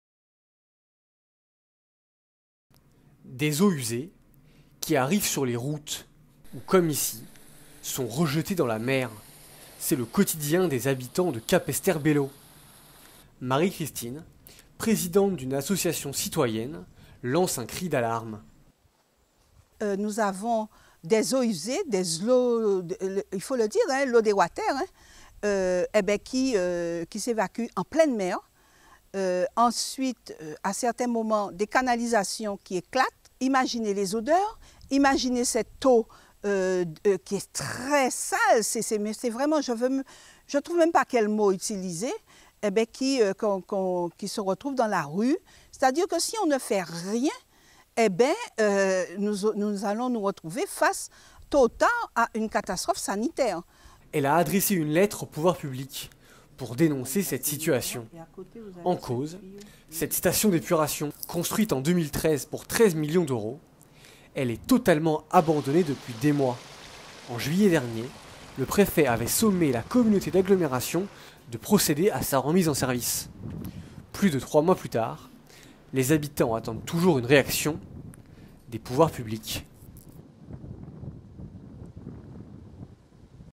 Le mauvais traitement des eaux usées à Capesterre-Belle-Eau
Les habitants de Capesterre-Belle-Eau mécontent du traitement des eaux usées. Elles envahissent les routes ou sont jetées à la mer...